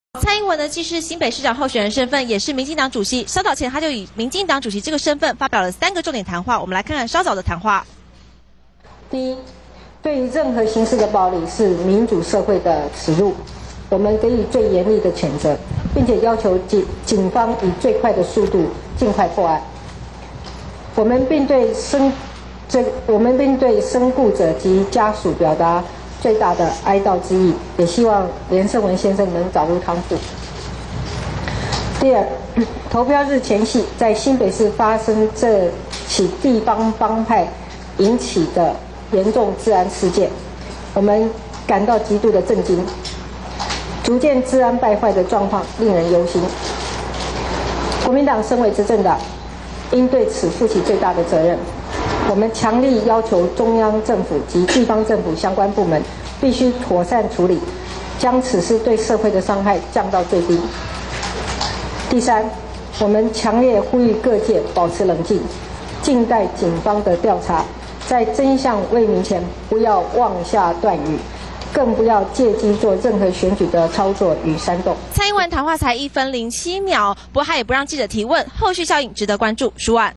连胜文助选时遭枪击 蔡英文称国民党应负责 （連勝文遭槍擊 ）
连胜文遭枪击受伤现场视频曝光 （連勝文遭槍擊现场视频）

连战之子连胜文遭枪击送医急救 嫌犯已被捕
[连胜文左脸被贯穿 没伤到脑部] [另一伤者生命垂危] [白岩松:连胜文遭枪击让人想起阿扁遭袭事件]
[马英九到台大医院探望连胜文 ][证实连胜文没有生命危险] [严厉谴责枪击行为] [表示全力缉凶]
[马英九证实连胜文没有生命危险] [严厉谴责] [表示全力缉凶] [胡志强为连胜文祈祷 提早终止造势晚会]连胜文助选遭枪击 连战含泪呼吁民众用选票回击
[顾不上儿子伤势 连战继续助选郝龙斌] [福建媒体准确预测选前之夜再现"两颗子弹"]国民党民代转述连胜文遭枪击过程 凶手开两枪
[开枪男子为台湾永和地区帮派分子] [两名嫌犯开枪有预谋 另一名在逃]